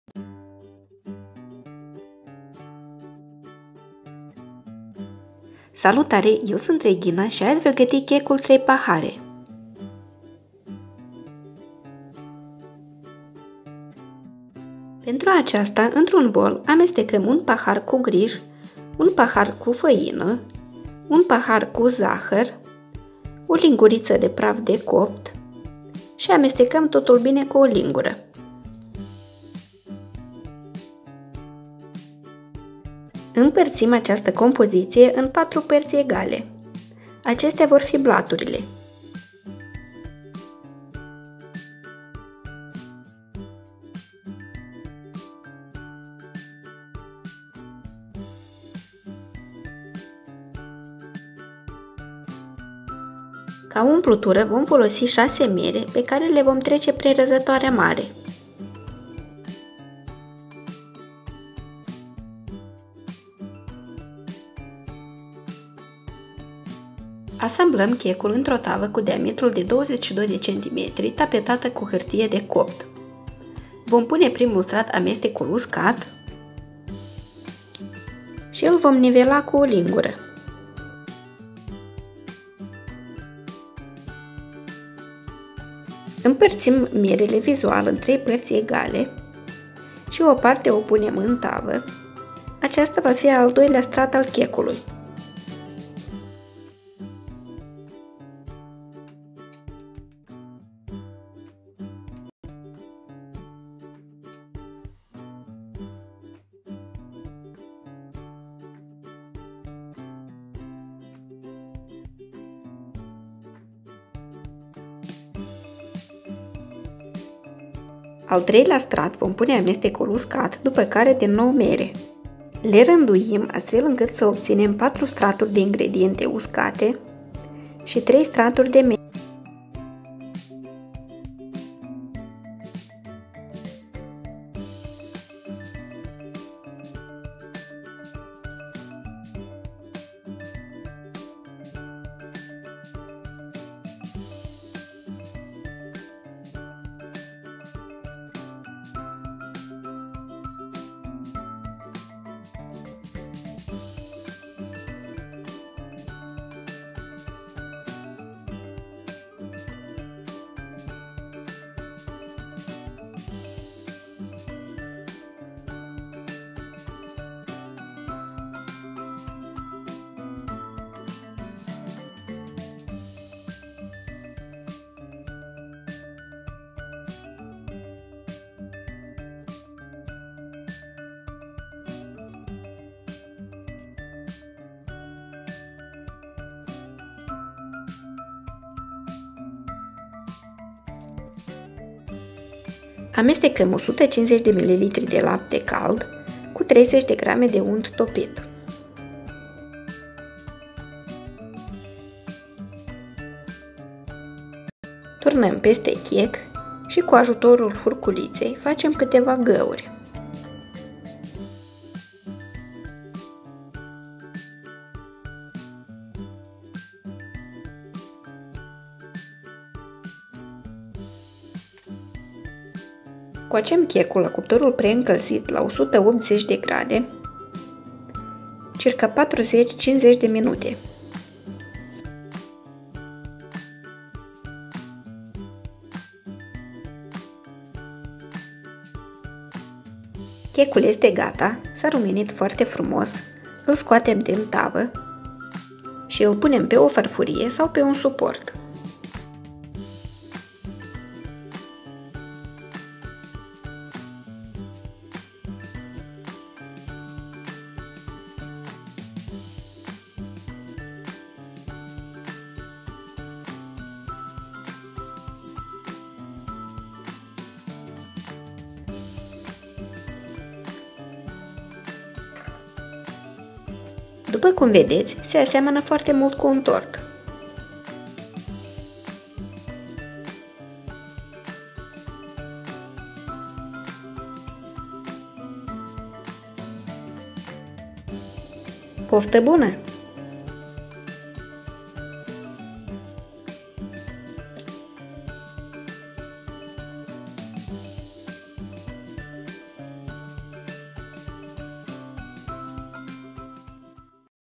Zici că-i tort , CHEC PRESURAT "3 PAHARE" | Reghina Cebotari
Ingrediente:          (1 pahar = 250 ml)
1 pahar Griș
1 pahar Făină
1 pahar Zahăr
1 linguriță praf de copt
6 Mere
150 ml. Lapte
30 gr. Unt